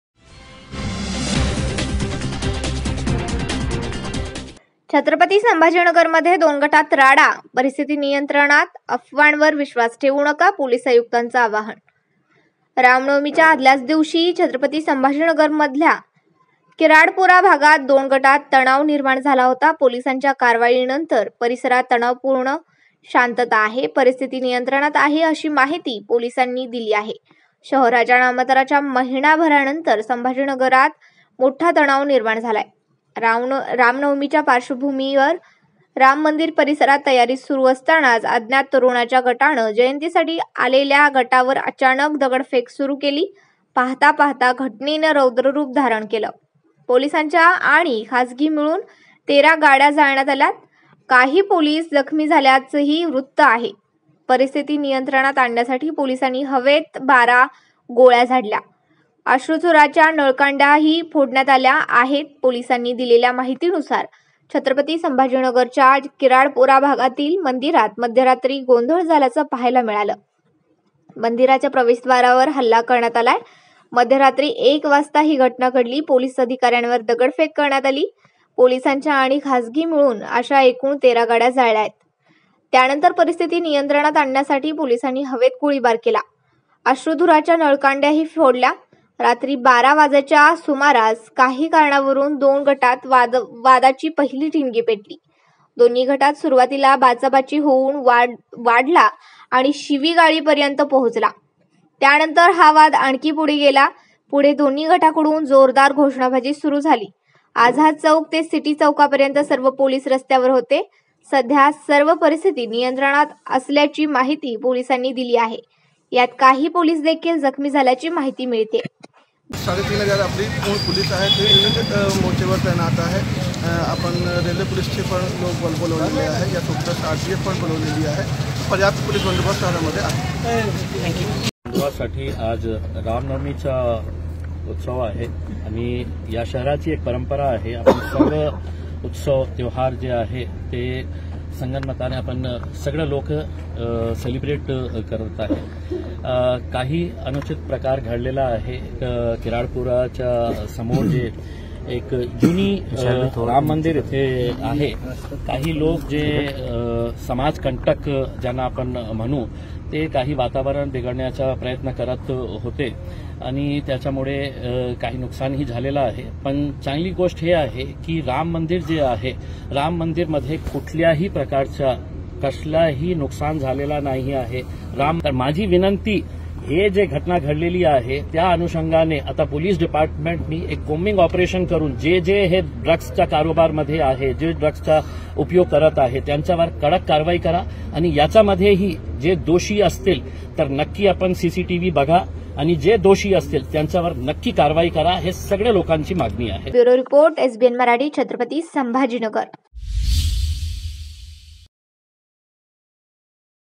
SAMBHAJINAGAR | छत्रपती संभाजीनगरमध्ये दोन गटांत राडा; परिस्थिती नियंत्रणात
छत्रपती संभाजीनगरमध्ये दोन गटांत राडा; परिस्थिती नियंत्रणात, अफवांवर विश्वास ठेवू नका, पोलीस आयुक्तांचं आवाहन